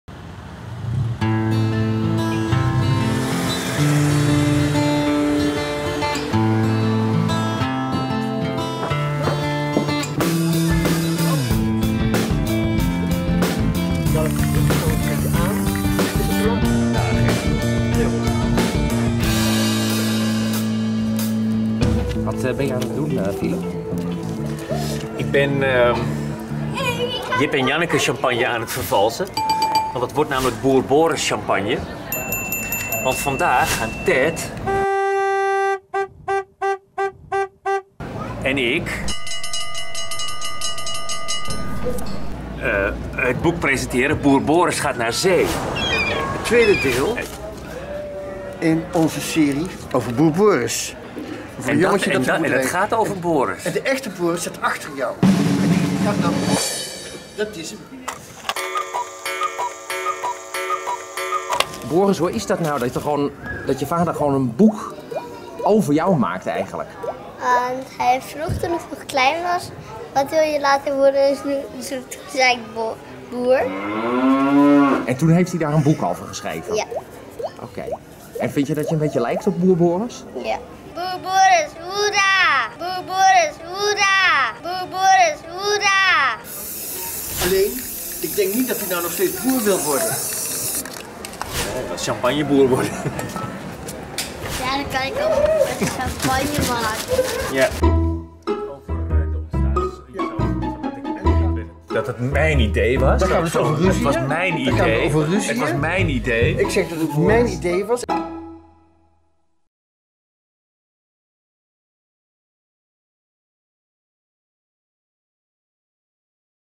Ted van Lieshout & Philip Hopman: Boer Boris gaat naar zee (1/3)
Deel 1! - Ted van Lieshout en Philip Hopman presenteren de opvolger van, 'Boer Boris', één van de "10 Beste prentenboeken van 2014": 'Boer Boris gaat naar zee'. Ze doen dat eind juni 2013 in boekhandel H. de Vries in Haarlem. Bart te Plate van Roelroel Media maakte deze film in drie korte delen. Dit is deel 1.